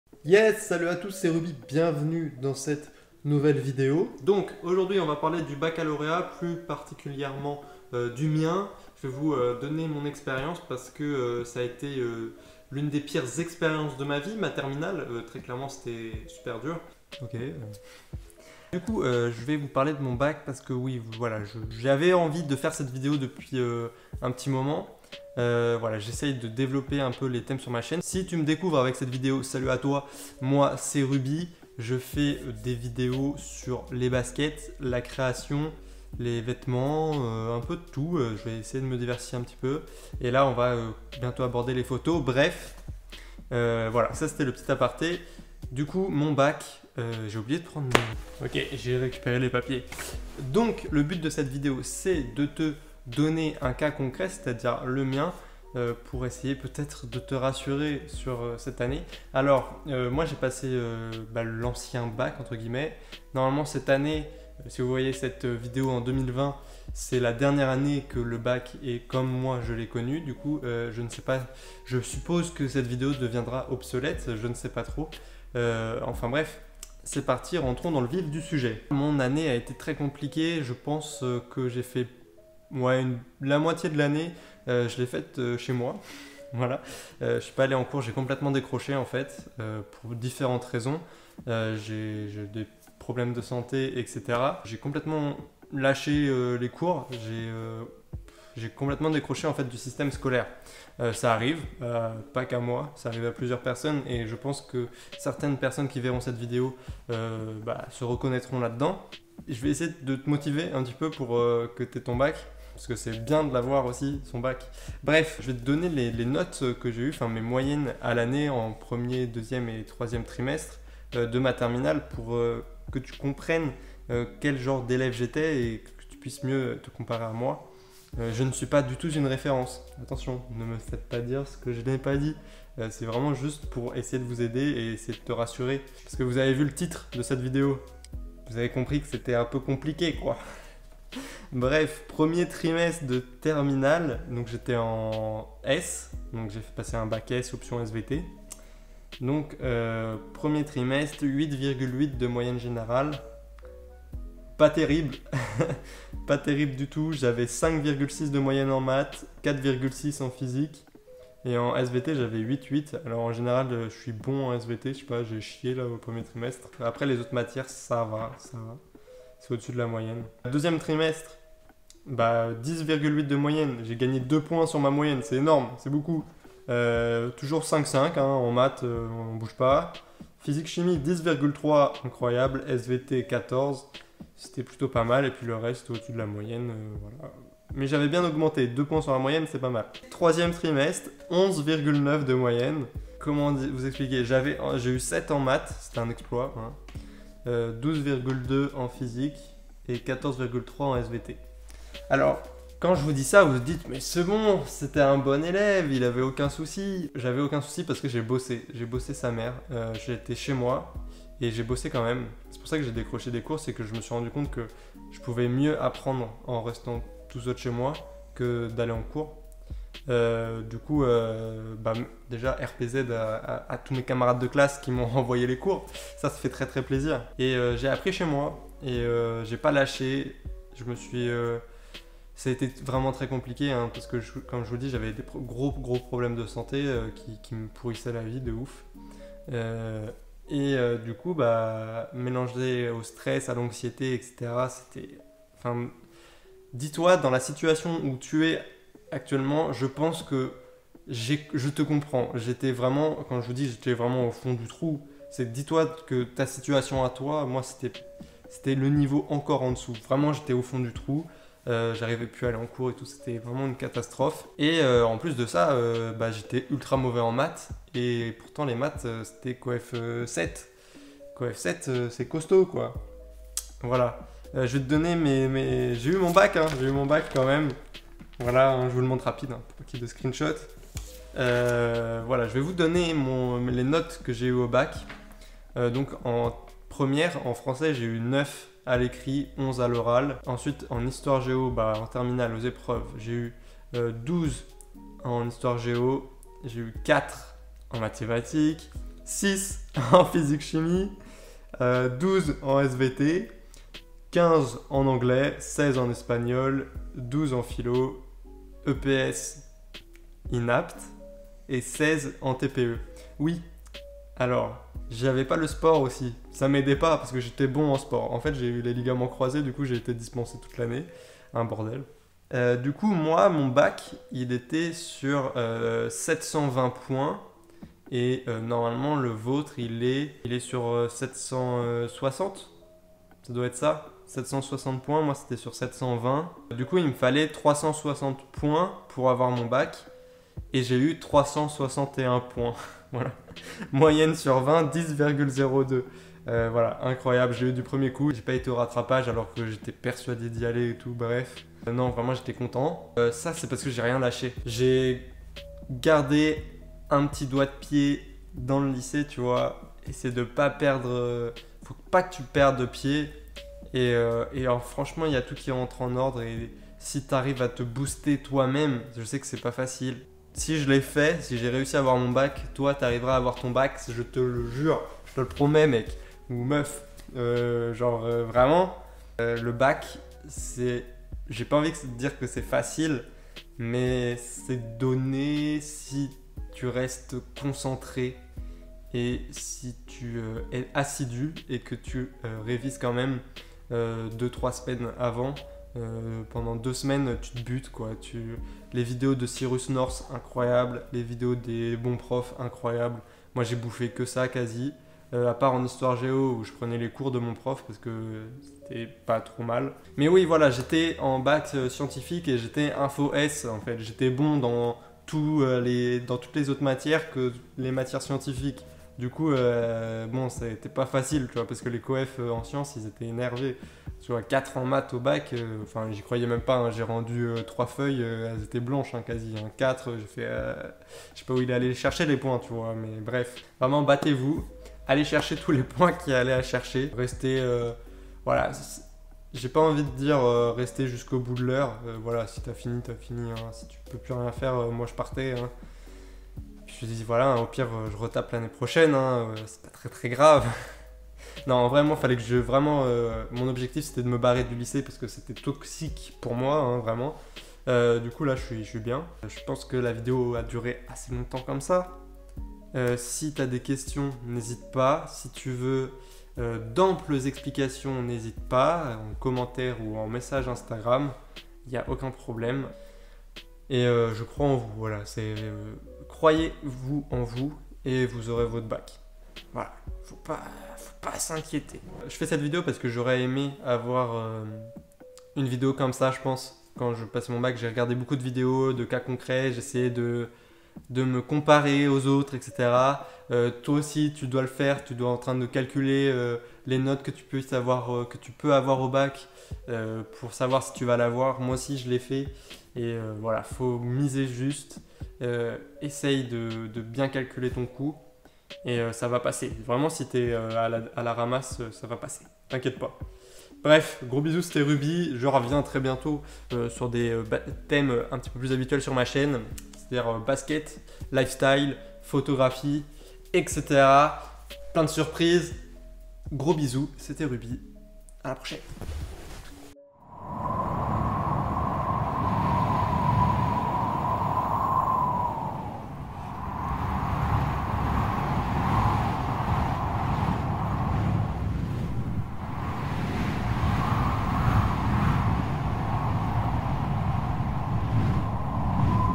MES NOTES DU BAC S 😅 (catastrophe)
Yes !

Salut moi c'est Rüby ! Dans cette vidéo je t'explique ma terminale et le fait que j'ai eu mon bac avec 4 en Maths et 6 en physique...
Avoir son bac avec 9 de moyenne c’est possible !

SYS !